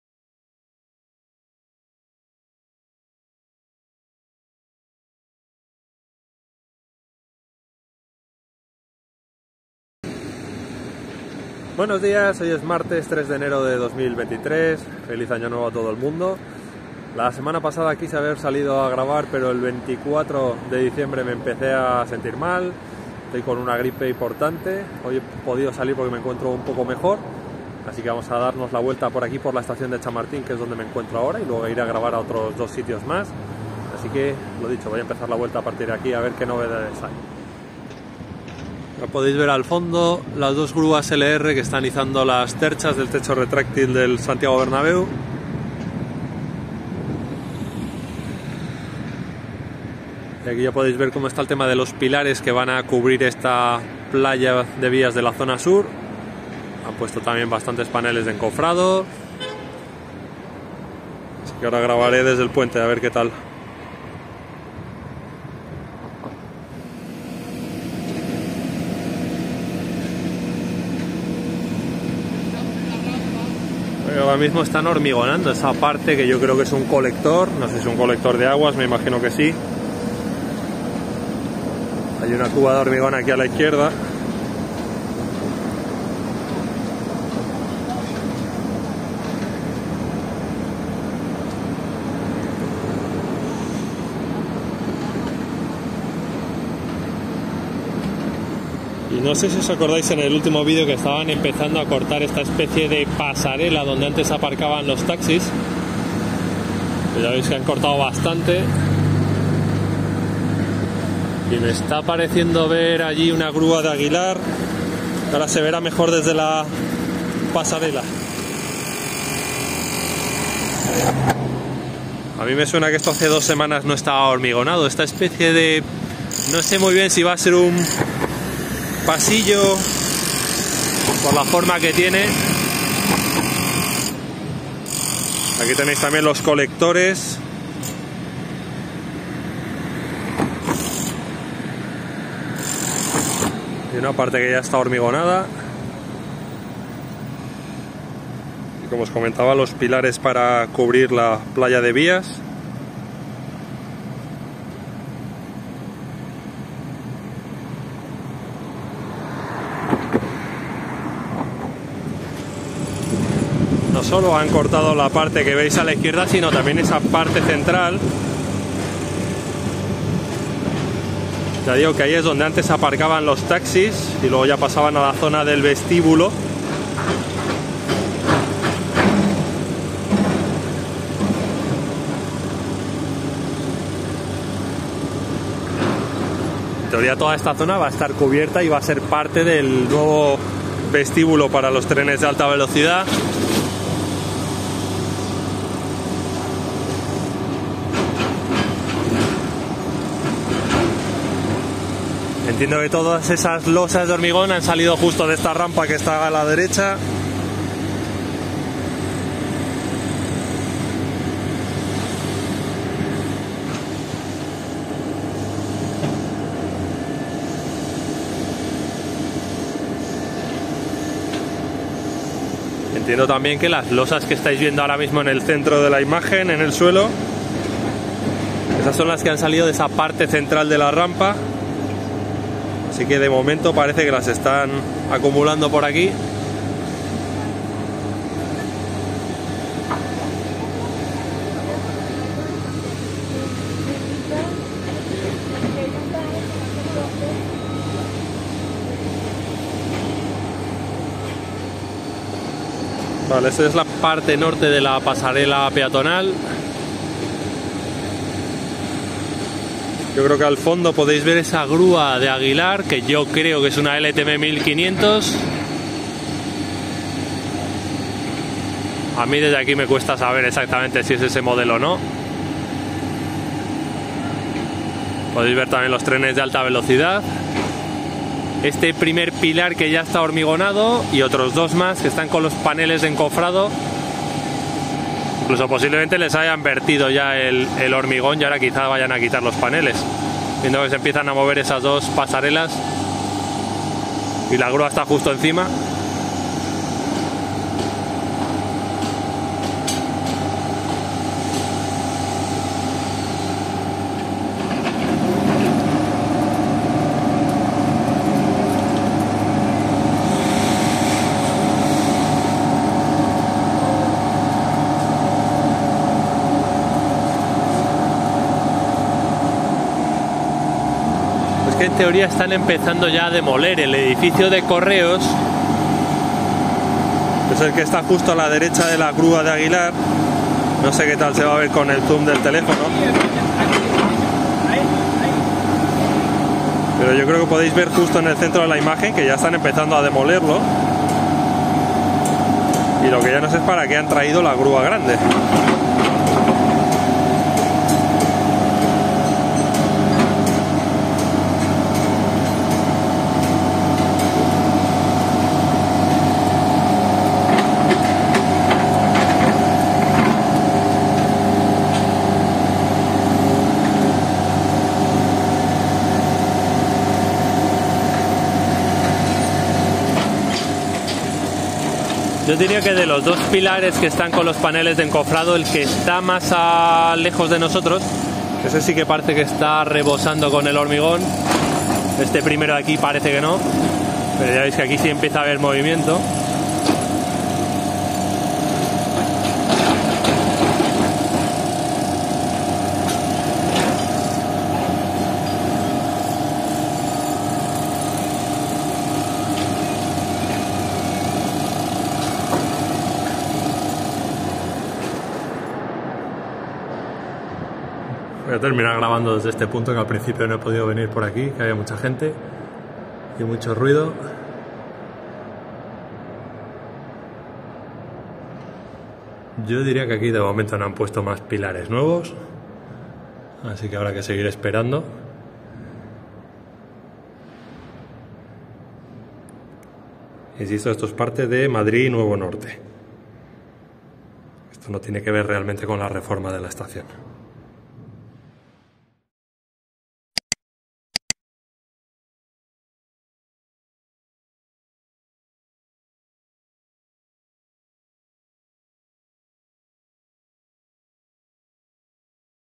🚧 PREPARANDO NUEVOS PILARES DEL VESTÍBULO Y DERRIBOS DEL EDIFICIO DE CORREOS | 3 DE ENERO DE 2022 🚧
Empezamos un nuevo año y seguimos avanzando en las obras de la estación de Chamartín con claros avances en la zona del vestíbulo. Sin embargo, creo que algo tan interesante como el derribo del antiguo edificio de Correos no se va a poder ver adecuadamente porque está en una zona donde no hay acceso. Al menos no que yo sepa.

Por allí había una grúa grande de Aguilar, quizás una LTM 1.500, que estaba cerca del mencionado edificio. No sé si está ayudando en las labores de demolición o si tiene alguna otra finalidad. Pienso si se podría ver algo desde los andenes que no son del AVE, es decir, a los que creo que puedo tener acceso sin billete. Es decir, a los andenes que están más hacia el oeste de la estación. Lo intentaré ver en futuras visitas.

Por lo demás, siguen completando pilares en la zona norte de la pasarela peatonal. Ya hay uno completamente terminado y dos que podrían estar incluso hormigonados, a falta de que retiren los paneles de encofrado. Siguen cortando y quitando la rampa donde antes estaban los taxis, aunque toda esa zona tiene que formar parte del futuro vestíbulo.

¡Feliz martes!